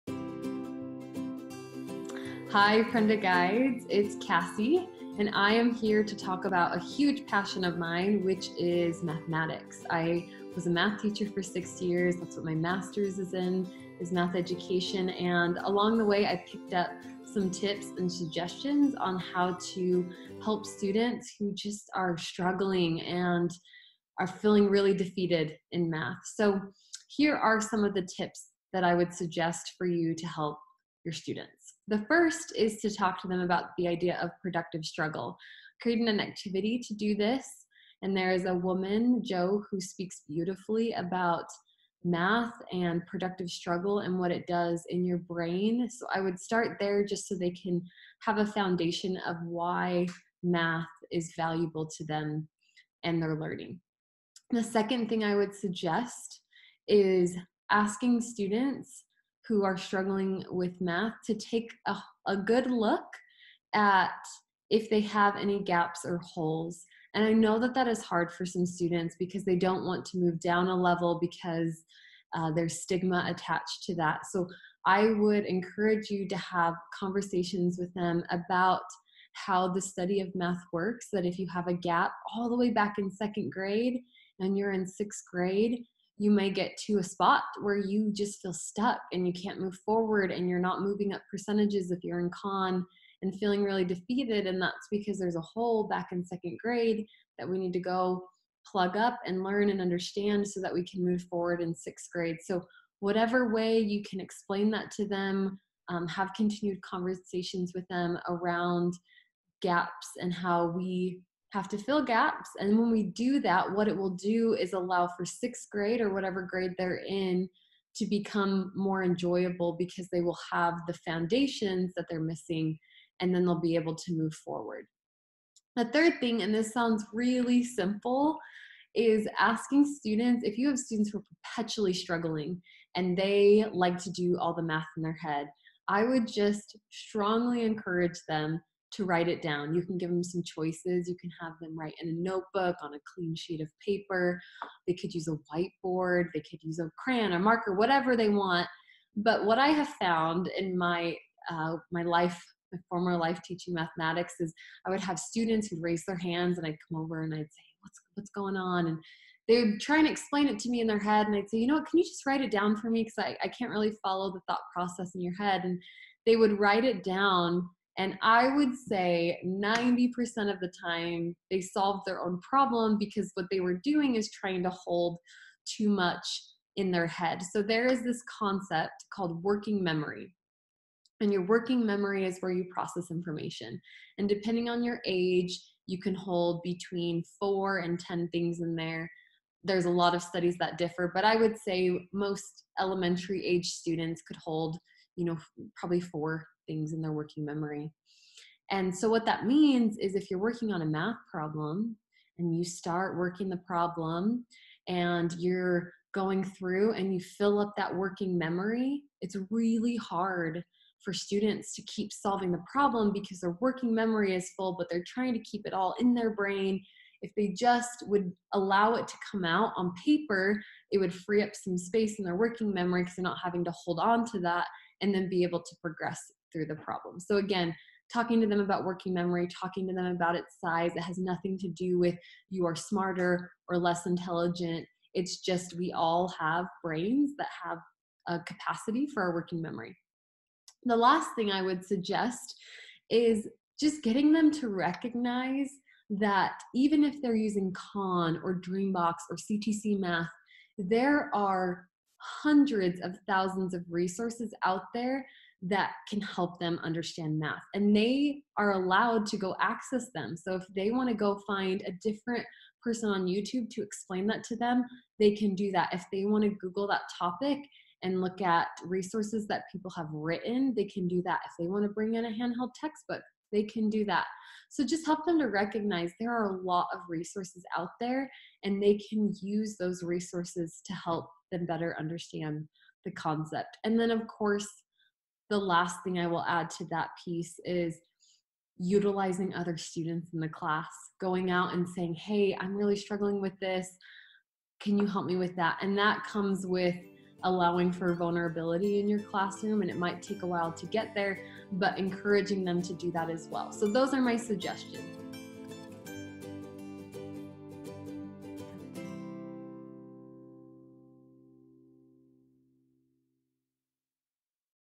Math Tips for Struggling Students
Tip #1: Teach students about productive struggle.
Tip #3: Write it down - encourage students to write down their steps and they work through math problems.
Tip #4: Encourage students to access outside resources to help them learn math topics (for example, other YouTubers, internet resources, textbooks, etc.)
*Favorite math YouTuber: PatrickJMT